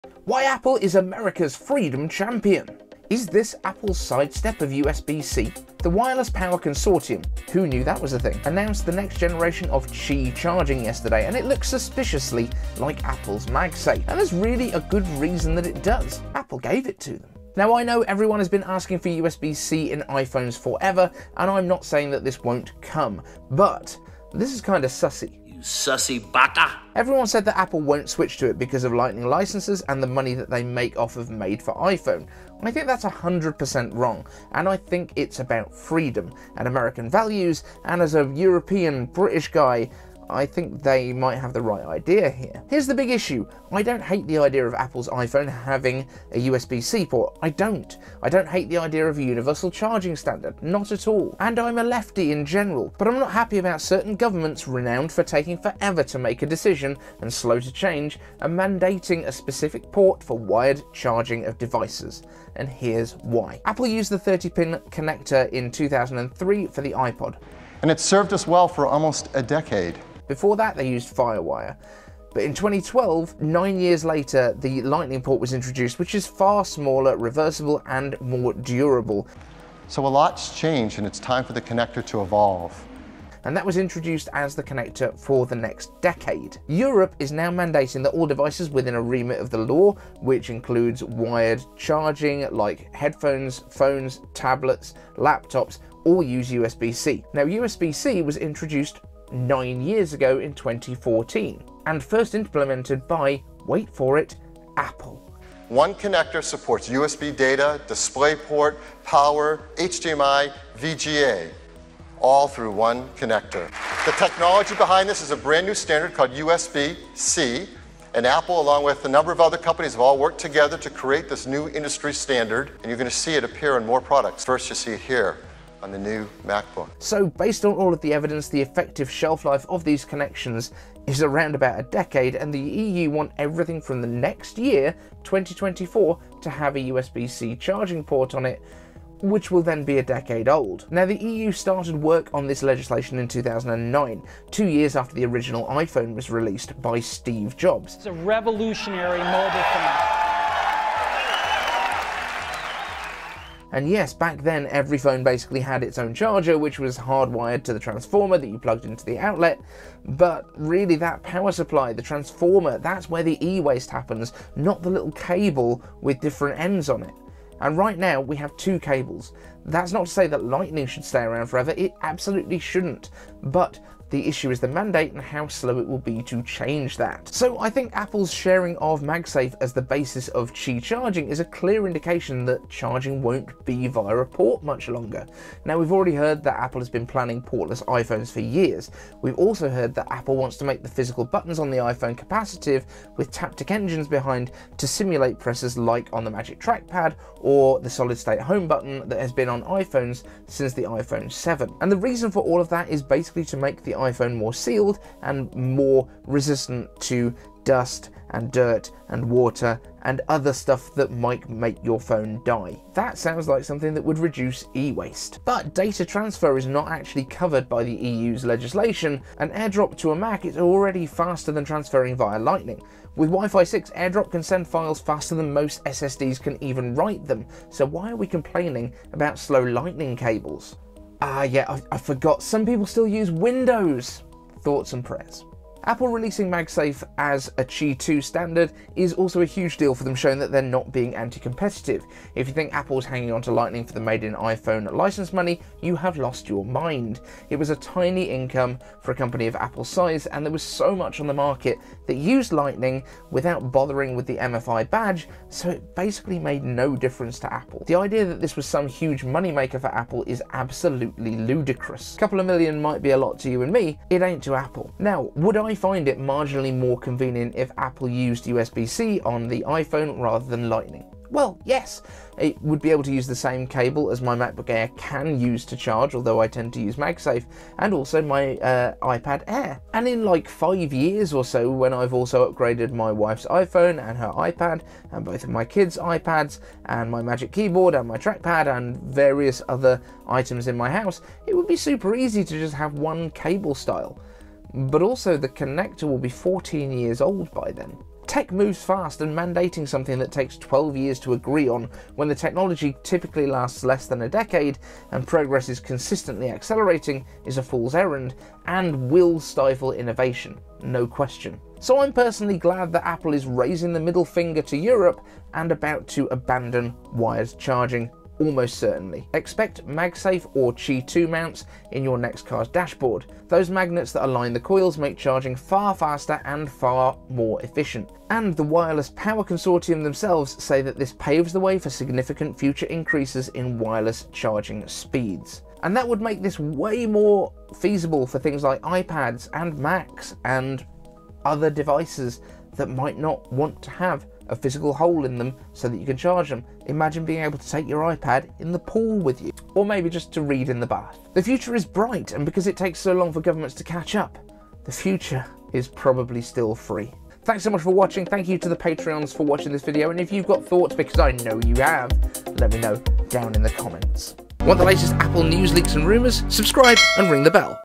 Apple's Middle Finger to Europe! 😳 Qi2 Open Wireless Charging!
Is this Apple’s sidestep of USB-C?

The Wireless Power Consortium (Who knew that was a thing?) announced the next generation of Qi charging yesterday, and it looks suspiciously like Apple’s MagSafe.

And there’s a really great reason that it does. Apple gave it to them.

Now I know, everyone has been asking for USB-C in iPhones forever, and I’m not saying that won’t come. BUT. This is kinda sussy.

Everyone said Apple won’t switch to it because of Lighting licenses and the money they make off Made for iPhone. I think that’s 100% wrong. I think it’s about freedom, and American values, and as a European British guy, I think they may have the right idea here.

Here’s the big issue. I don’t hate the idea of Apple’s iPhone having a USB-C Port. I don’t. I don’t hate the idea of a universal charging standard. Not at all. I’m not happy though that a certain government, renowned for taking forever to make a decision and slow to change are mandating a specific port for wired charging of devices. And here’s why.

Apple used the 30 pin connector in 2003 for the iPod. Before that, they used firewire. In 2012, nine years later, the far superior Lightning port was introduced, which is far smaller, reversible and more durable. That was introduced as the connector for the next decade.

Europe is now mandating that all devices within the remit of the law with a wired connection including headphones, phones, tablets, laptops and more use USB-C. USB-C was introduced 9 years ago, in 2014. And first implemented by, wait for it… Apple, in the 12” MacBook.

So based on the evidence, the effective shelflife of these connectors is around a decade, and the EU wants everything from NEXT YEAR, 2024 to have USB-C charging, a standard which is a decade old.

The EU Started work on this legislation in 2009. Two years after the original iPhone was released. And yes, back then, every phone basically had its own charger, which was hard wired to the transformer you plugged into the outlet. But, really, that power supply, the transformer is where the e-Waste happens, not that little cable. And right now, we have 2 cables. That’s not to say Lightning should stay around forever. It shouldn’t. The issue is the mandate, and how slow that will be to change.

So I think Apple’s sharing of MagSafe as the basis of Qi2 is a clear indication that changing won’t be via a port for much longer. We’ve already heard that Apple has been planning portless phones for years. We’ve heard that Apple wants to make the physical buttons on the iPhone capacitive, with Taptic engines behind to simulate presses like the Magic Trackpad or the solid state home button on iPhones since the iPhone 7.

But data transfer is not covered by the EU’s legislation, and AirDrop to a Mac is already faster than transferring via lighting. With WiFi 6 AirDrop can send files faster than most SSDs can write them, so why are we complaining about slow lighting cables?

Oh, I guess some people still use Windows. Thoughts and Prayers.

Apple releasing MagSafe as Qi2 is also a huge deal for them showing they’re not anti competitive. If you think Apple was hanging onto Lighting for the Made for iPhone license money, you’ve lost your mind. It was a tiny income for a company of Apple’s size, and there was so much on the market using the lightning connection without bothering with the MFI Badge that it made no real difference. The idea this was some huge money maker for Apple is Ludicrous.

Would I find it marginally more convenient if Apple used USB-C on the iPhone than lightning? Well, yes, it would use the same cable as my MacBook Air *CAN* charge with and my iPad Air. And in 5 or so years, when we’ve also upgraded my wife’s iPhone and iPad, both my kid’s iPhones, my Mac Keyboard and trackpad and numerous other items in the house, it would be super easy to have just one cable style. But also, the connector will be 14 years old by then.

Tech moves fast, and mandating something that takes 12 years to agree on when the technology typically lasts a decade, and progress is constantly accelerating is a fool’s errand, and WILL stifle innovation. No question.

So I’m kind of glad Apple is raising the middle finger and probably about to abandon wired charging. Expect MagSafe, or Qi2 mounts on your next car’s dashboard. The magnets aligning the coils make charging FAR faster and FAR more efficient. And, the Wireless Power Consortium says this paves the way for “Significant future increases in wireless charging speeds”. The Future is bright, and because it takes so long for governments to catch up, the future, is FREE.